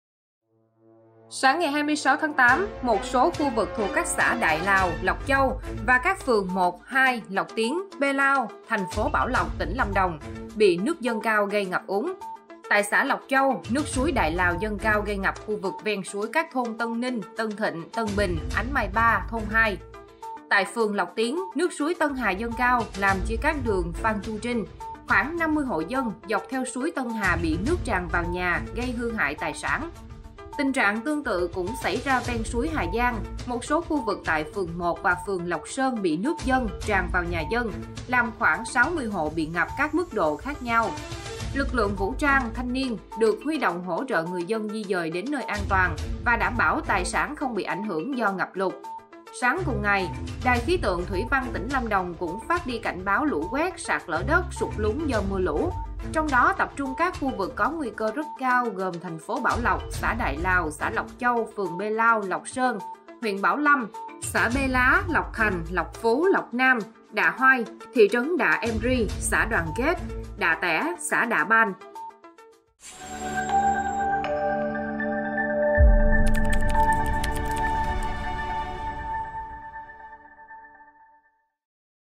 TP Bảo Lộc ngập nặng sau mưa lớn
Sáng 26-8, một số khu vực thuộc các xã Đại Lào, Lộc Châu và các phường 1, 2, Lộc Tiến, B’Lao, TP Bảo Lộc (tỉnh Lâm Đồng) … bị nước dâng cao gây ngập úng.